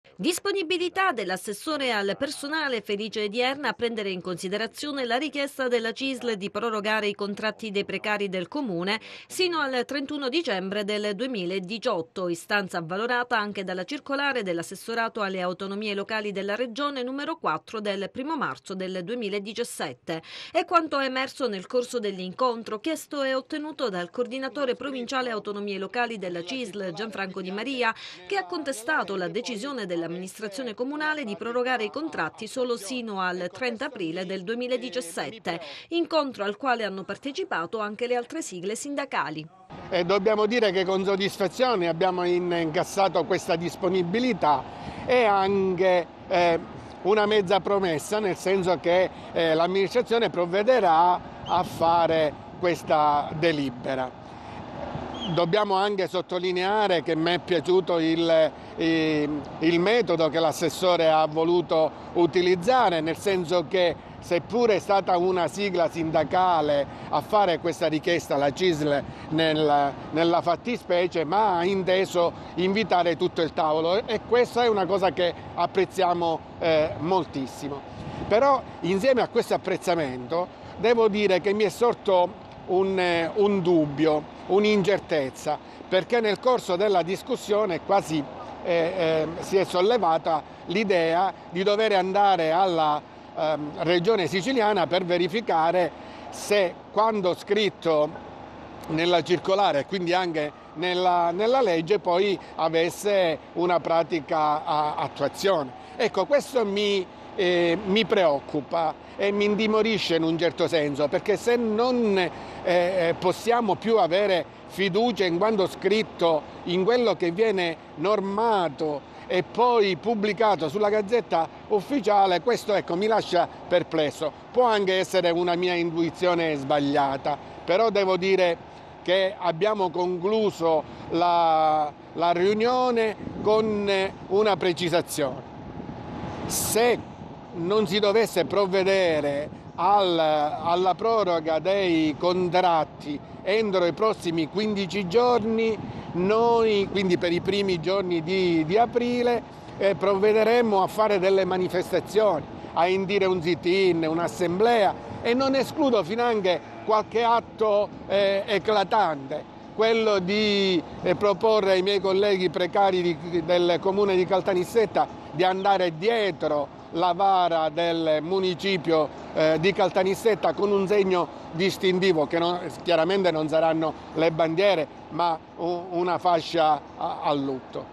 Comune, probabile proroga dei contratti
Incontro tra sindacati ed assessore al personale del comune per la proroga dei contratti dei dipendenti precari, ancora nulla di certo